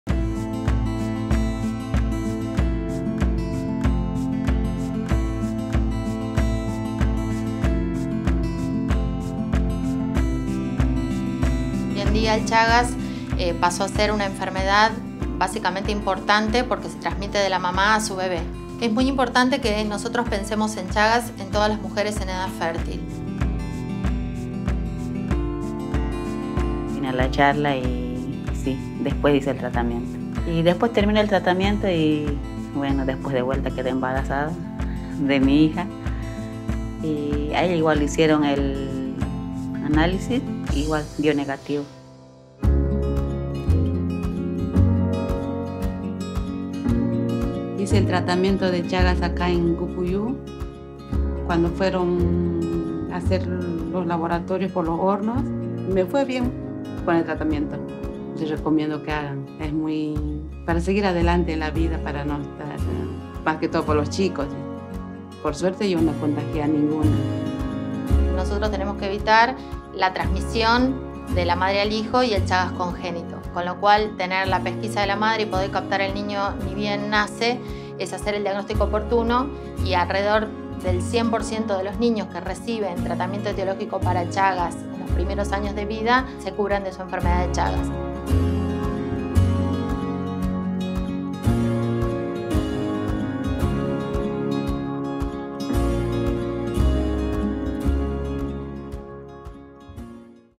Ningún bebé con Chagas
En el marco del Día Mundial de la Enfermedades Desatendidas, los países Iberoamericanos comprometidos en la lucha contra el chagas reafirman su compromiso para seguir brindando acceso al diagnóstico y tratamiento de la enfermedad a una población estimada en 5,5 millones de personas, de las cuales alrededor de 1,2 son mujeres en edad fértil.